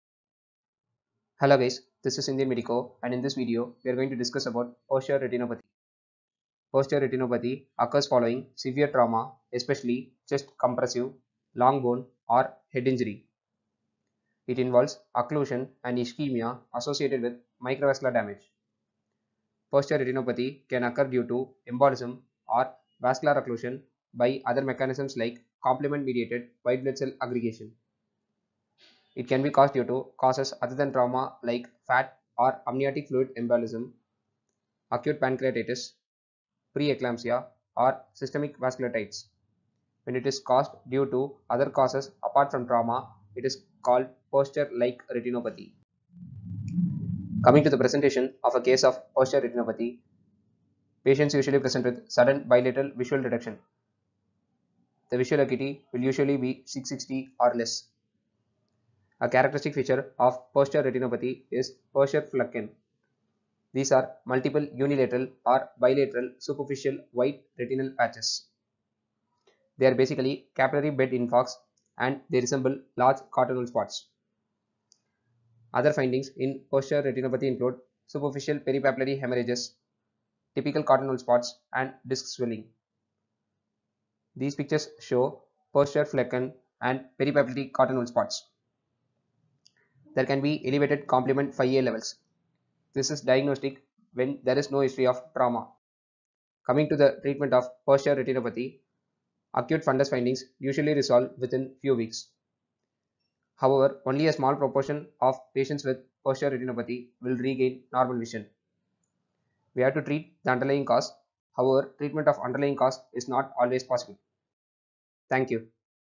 Purtscher Retinopathy - For Ophthalmology Residents
Video on 'Purtscher retinopathy' from the chapter 'Retinal vascular diseases' meant for ophthalmology postgraduates.